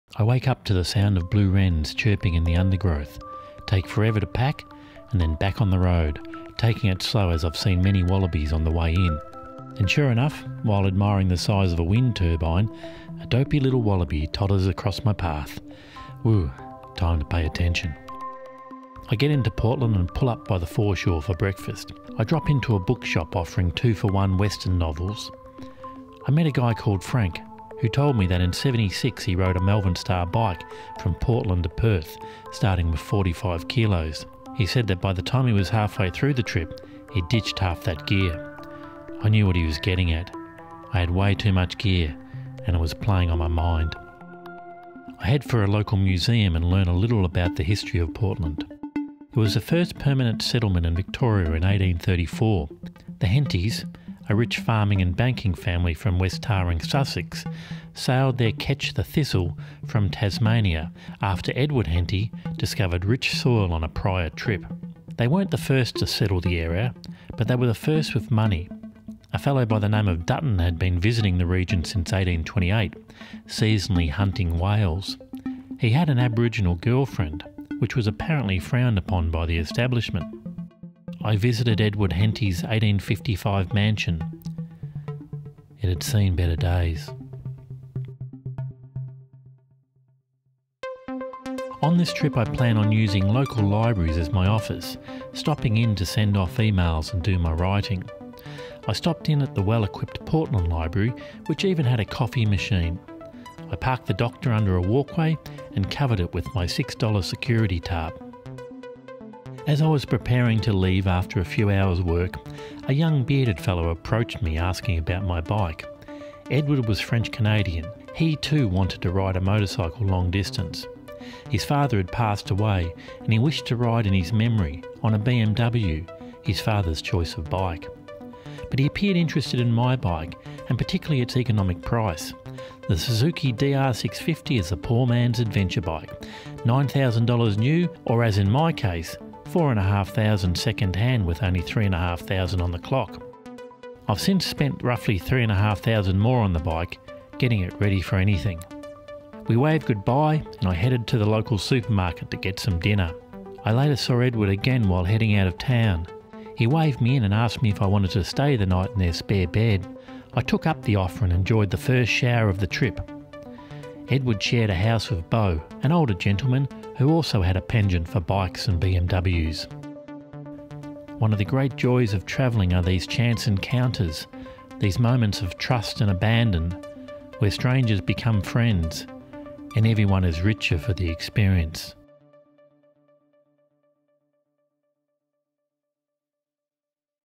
Australian Motorcycle Roadtrip Ep4
First bush camp and the kindness of strangers.

All footage copyright of overlander.tv and not to be used without prior permission